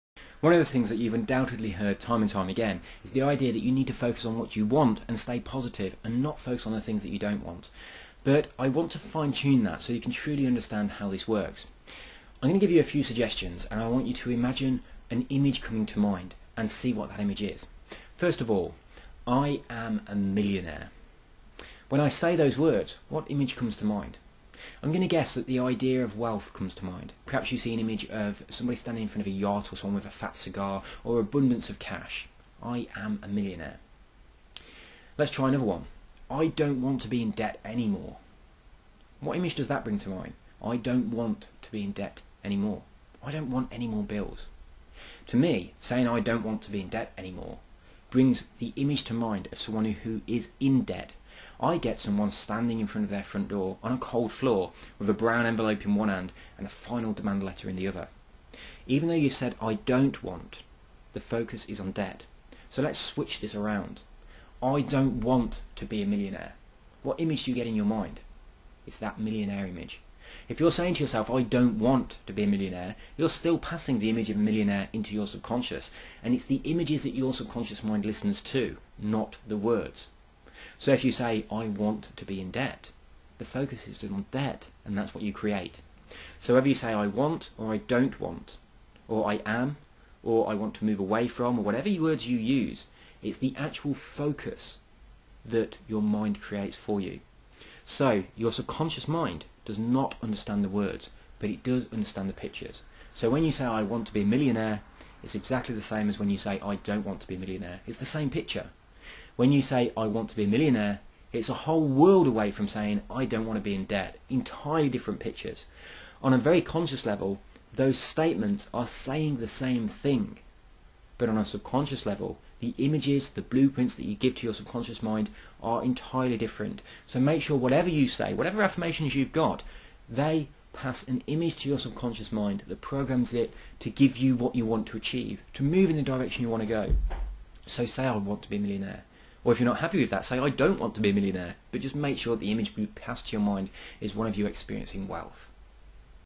Law of Attraction and Cosmic ordering tips - focus
The Law of Attraction brings you more of what you focus on, so it's important to focus on (and move towards) what you want... not what you don't want. This video reveals how you can focus on your desire in a way that will cause the law of attraction to make it manifest in your life, while avoiding the self sabotage often takes you in the exact opposite direction.